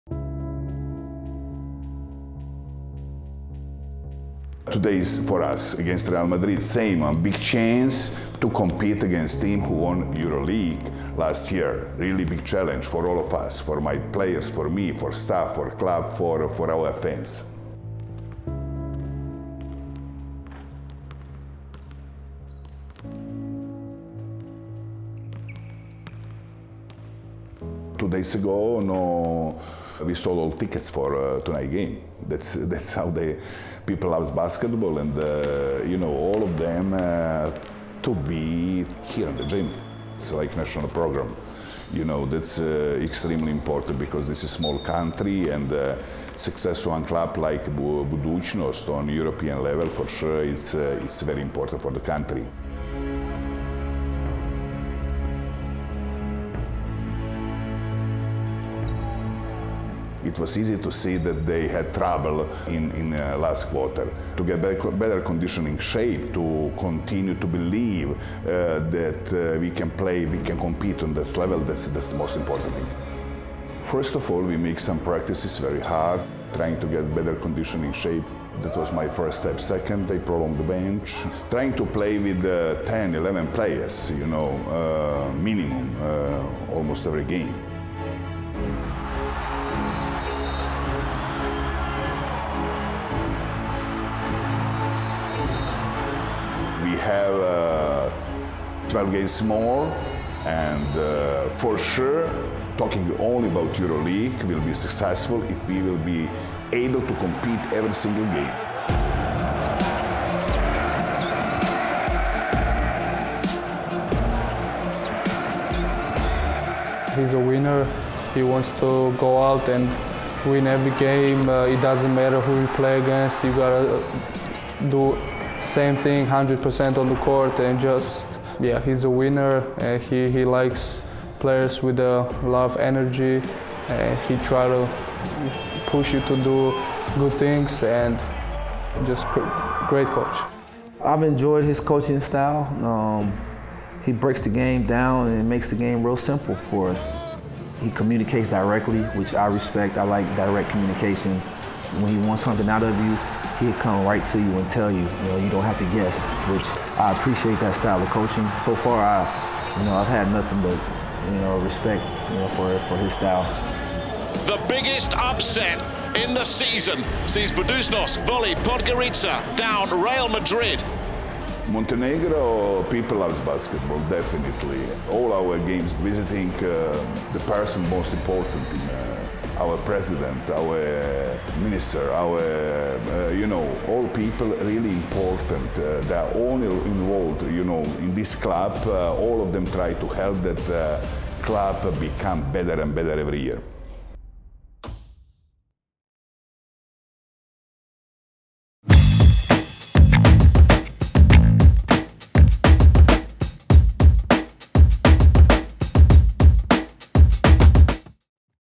Focus on: Jasmin Repesa, Buducnost VOLI Podgorica
Four games into his tenure at Buducnost VOLI Podgorica, the fingerprints of head coach Jasmin Repesa were all over the team's 73-60 upset of defending champion Real Madrid in Round 19.